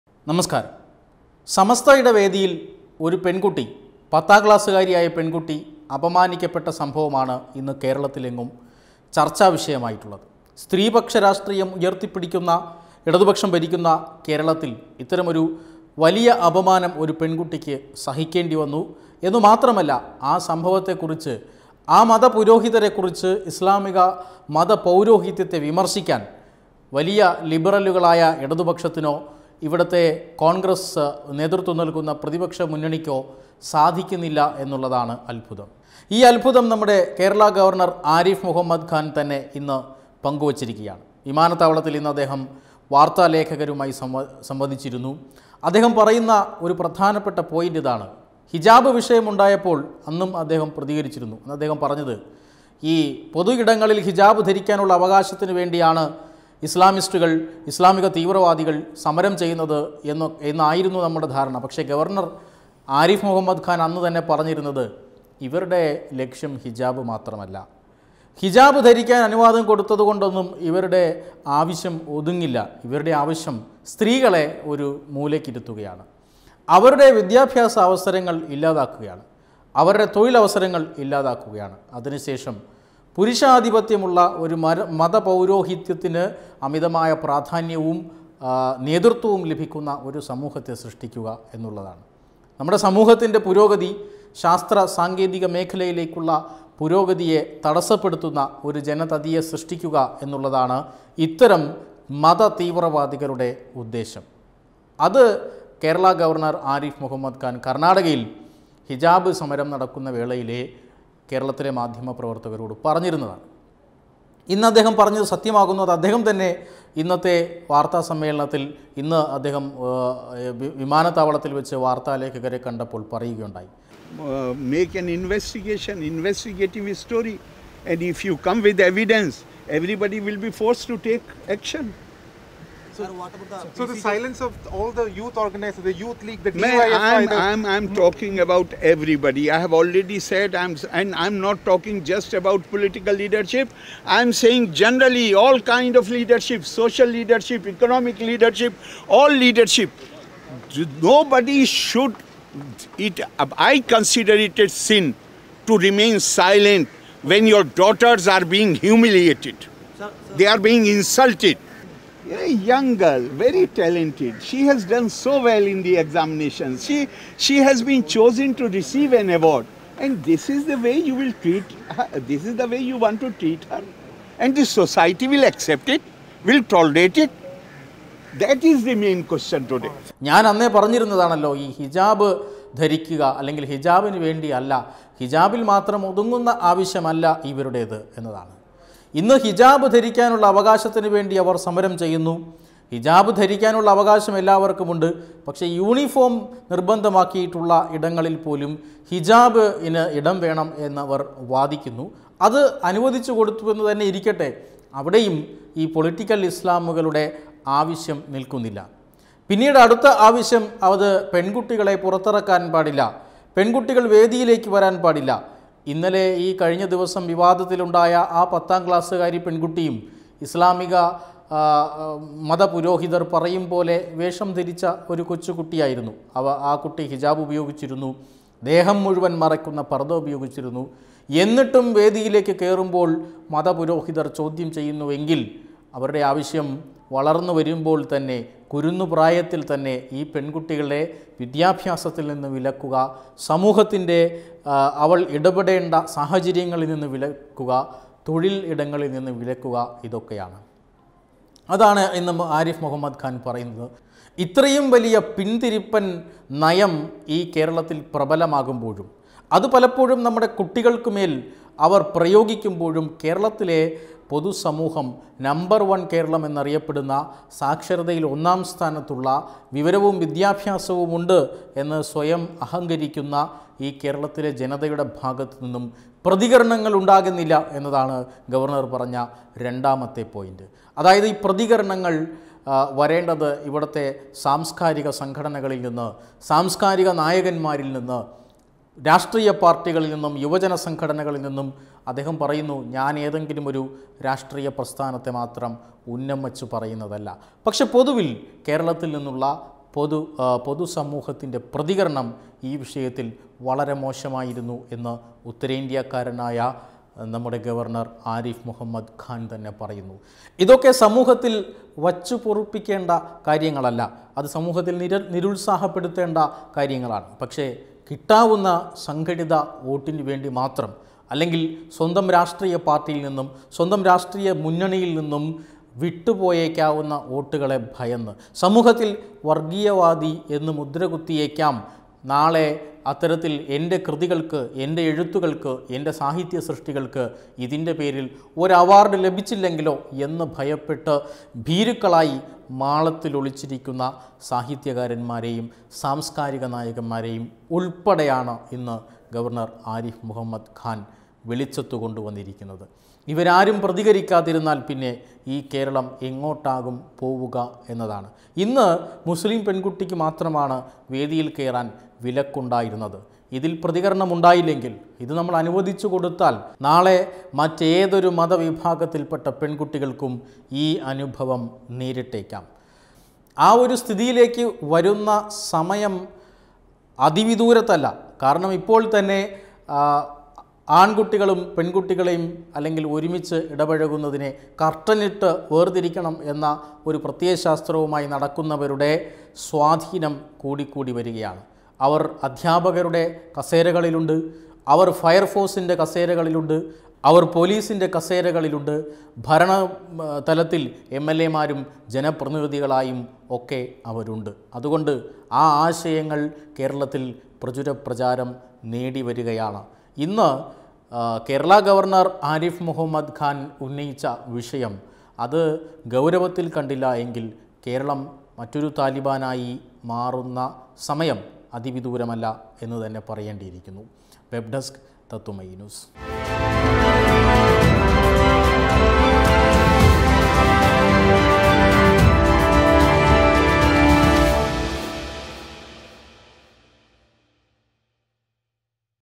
സമസ്ത ഉസ്താദിനെതിരെ ആഞ്ഞടിച്ച് ആരിഫ് മുഹമ്മദ് ഖാൻ | AARIF MOHAMMED KHAN
പെൺകുഞ്ഞുങ്ങൾ അപമാനിക്കപ്പെട്ടിട്ടും പ്രതികരിക്കാത്ത കേരള സമൂഹം നാണക്കേടെന്ന് ഗവർണർ